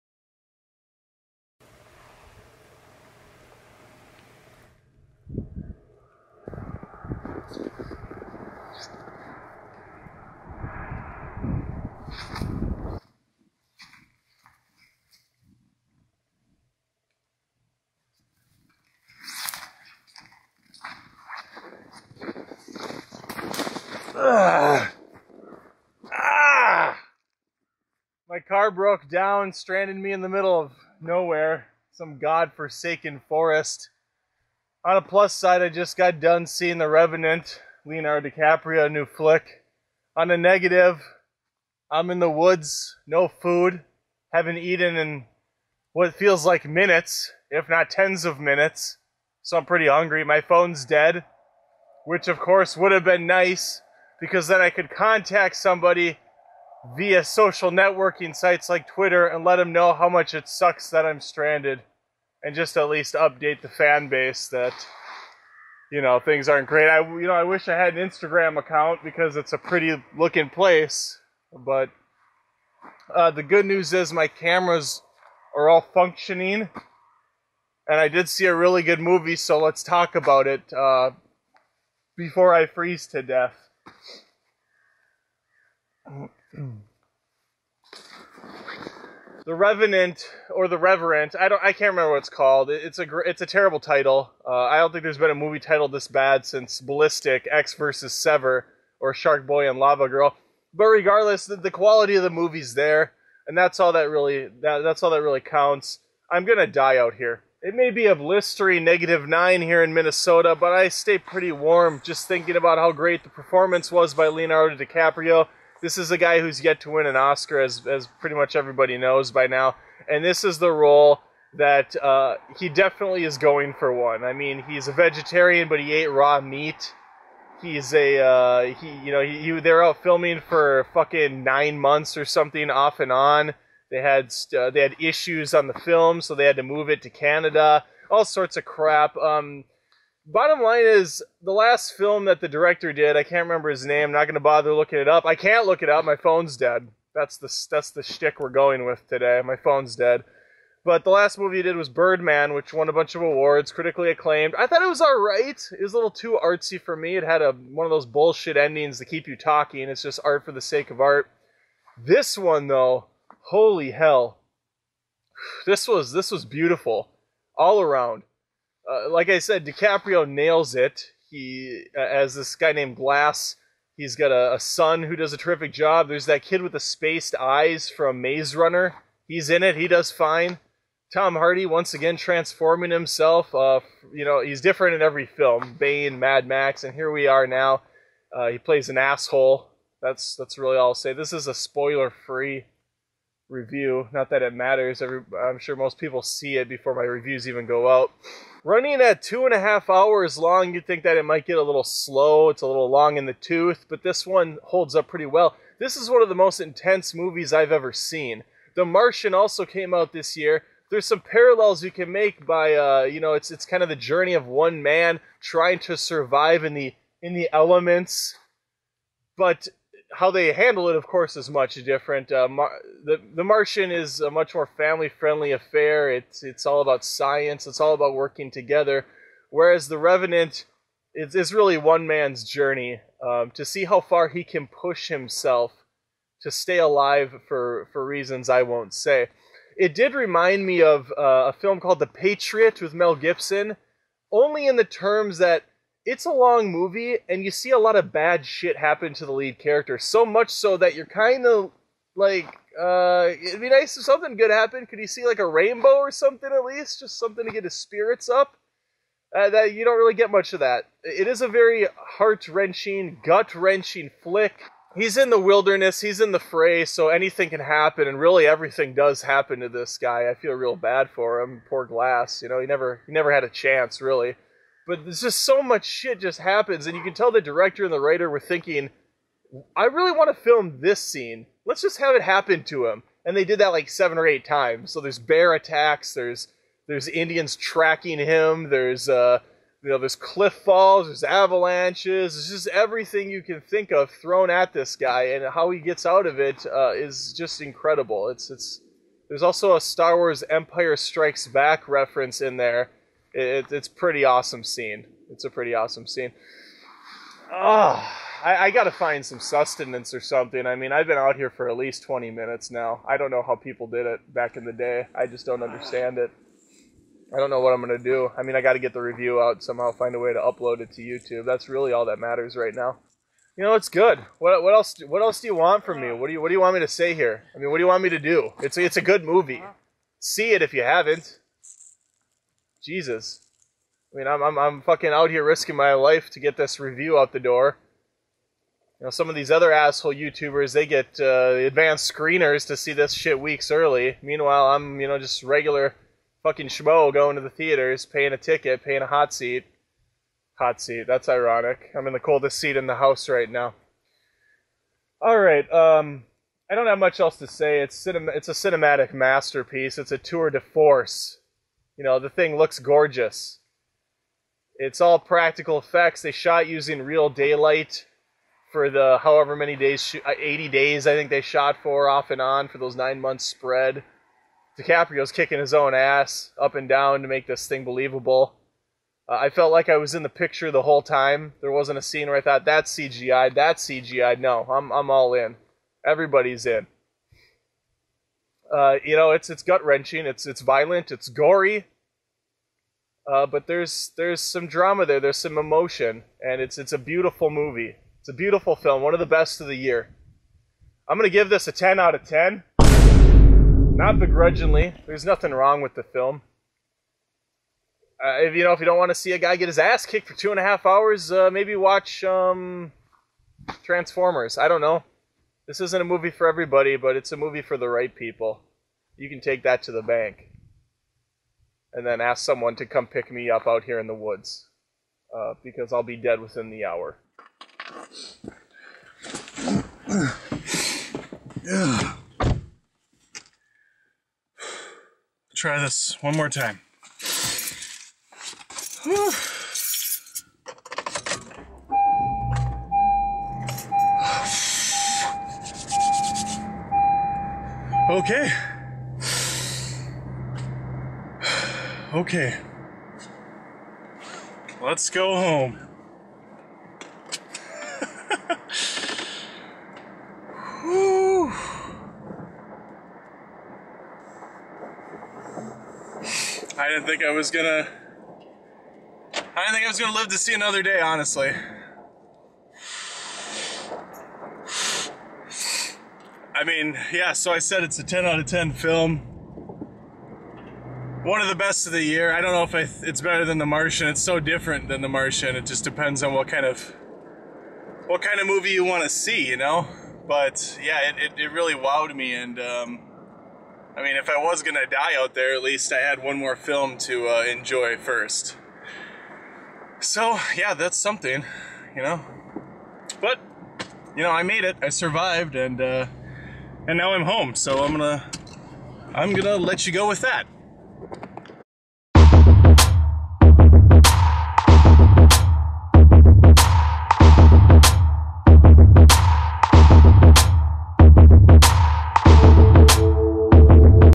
The Revenant Review : Carside Reviews ep31
Stranded alone in the middle of nowhere. No food, no water, no phone reception. This doesn't stop Adam from giving his 3-camera review of The Revenant. An intense, gritty film with Leonardo DiCaprio once again proving why he's one of the best actors in the business. The Revenant is one of the best films of 2015 and Adam breaks down some reasons why on Carside Reviews.